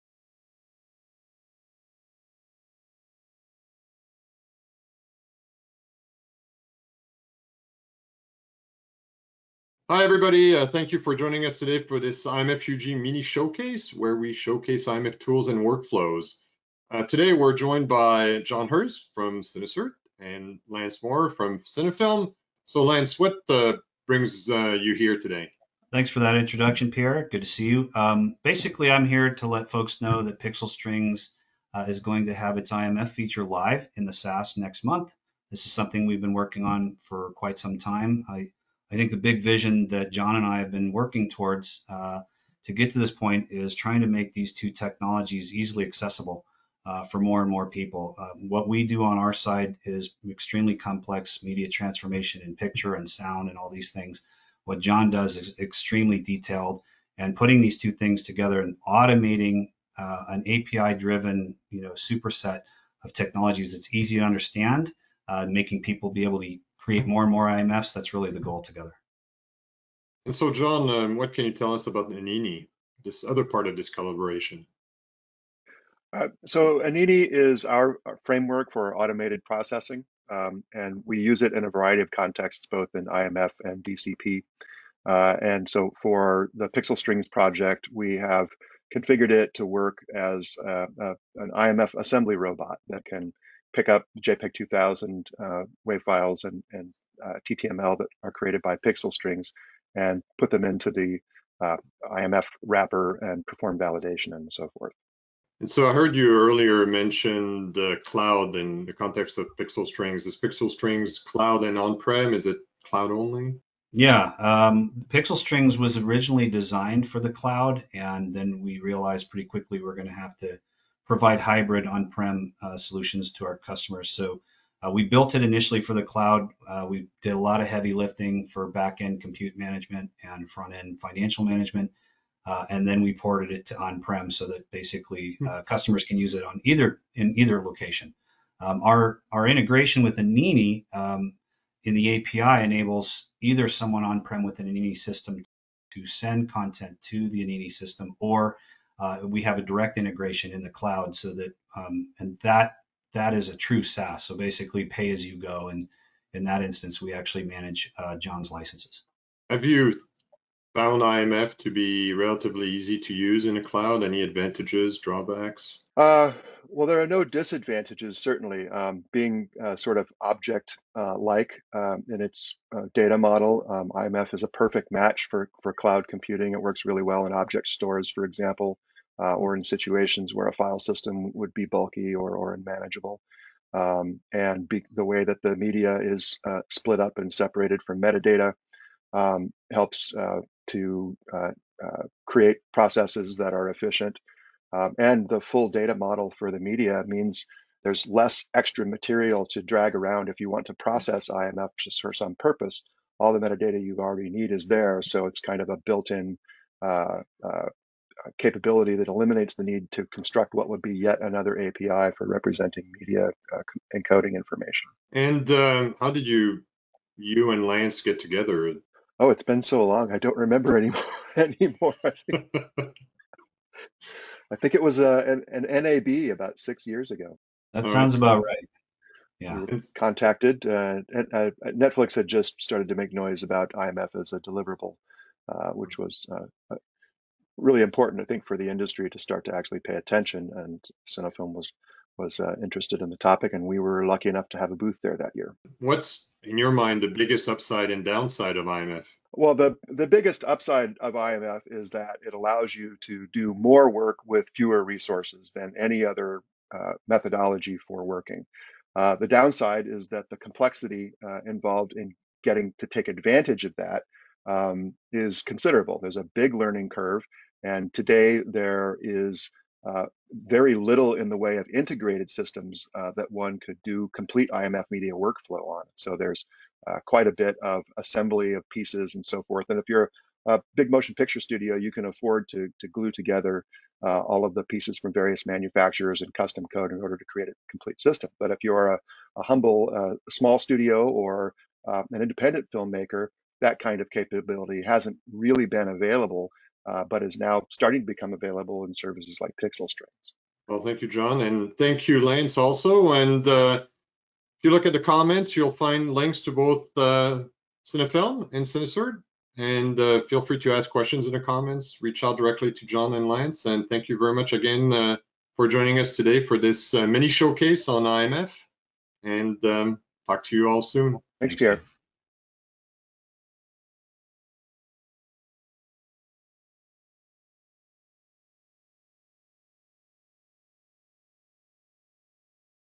Cinnafilm and CineCert — IMF Mini-Showcase
Lance Maurer (Cinnafilm) and John Hurst (CineCert) tell us about their recent collaboration that combines Cinnafilm's Pixel Strings and CineCert's Anini into a cloud-based IMF processing engine.